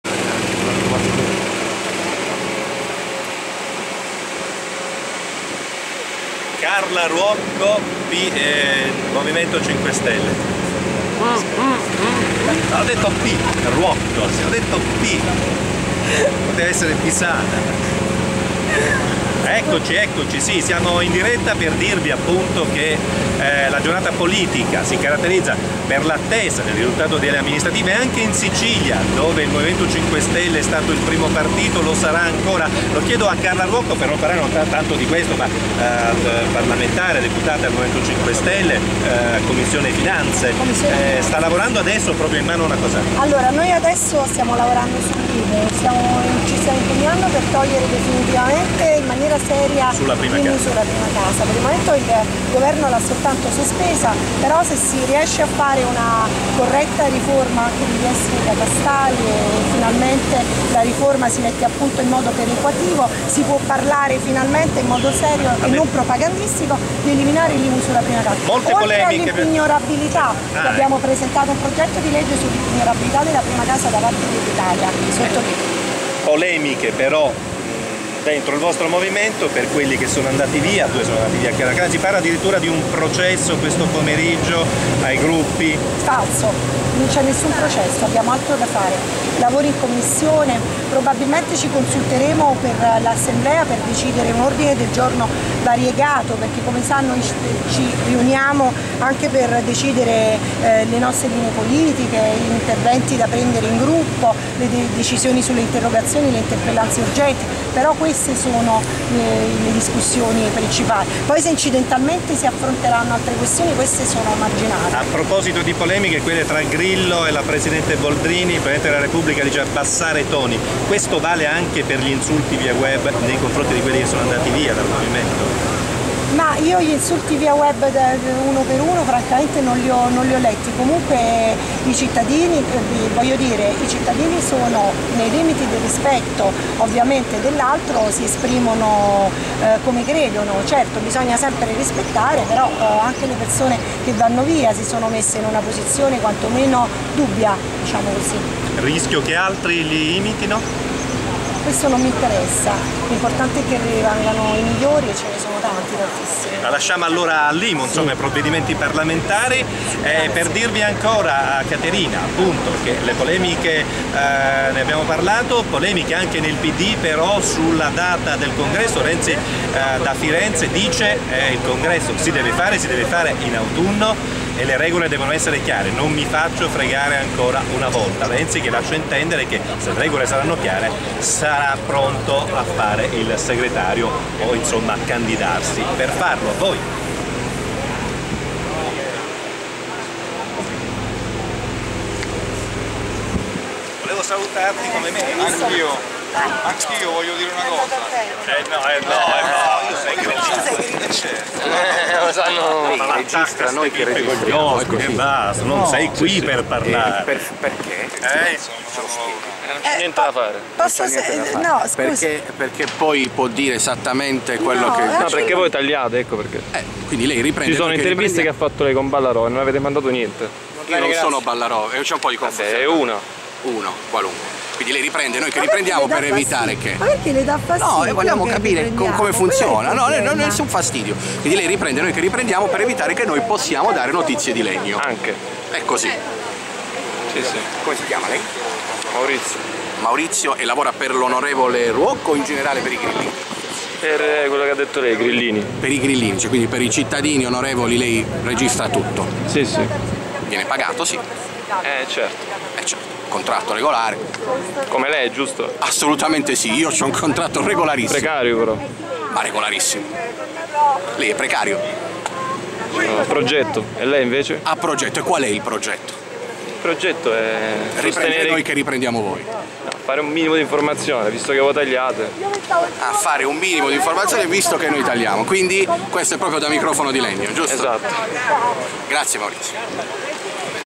10/6/2013 L'intervista sull'intervista (con polemica)
Dopo l'intervista in diretta al Tg4 da parte di Carla Ruocco, c'è chi polemizza con Maurizio Argentieri dell'ufficio comunicazione per le sue riprese integrali.